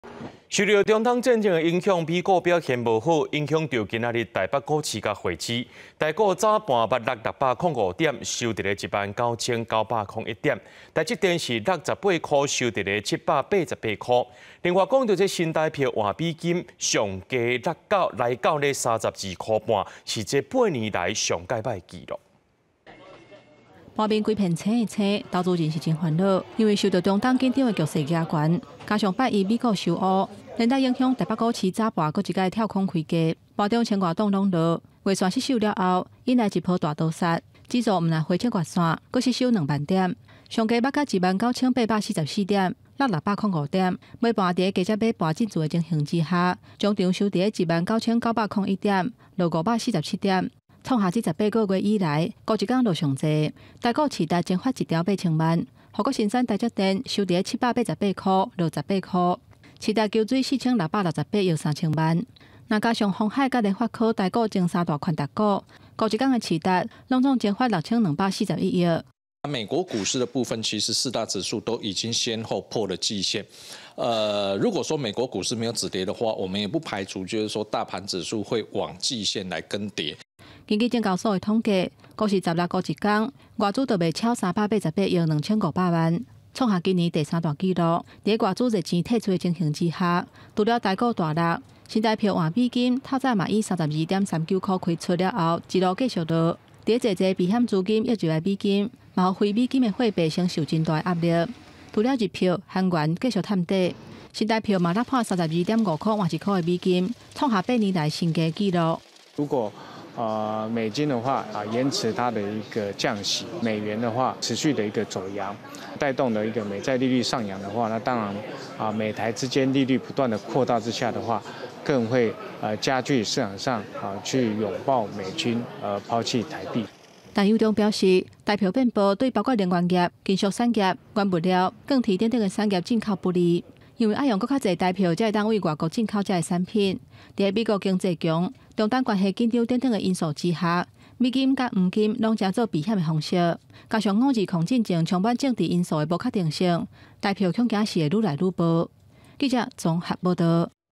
中東緊張、美股落 臺股跋超過500點、2萬點失守 | 公視台語台 | 20240416
受著中東戰爭的影響，美股表現無好，影響著今仔日的台北股市、匯市。台股早盤捌落605點，落尾收佇19901點，台積電落18箍，收佇788箍。另外，新台票換美金上低落到32箍半，是這8年來上䆀的紀錄。

四節新聞 ：　逐工（每天）09:00 / 12:00 / 15:00（拜一至拜五） / 19:00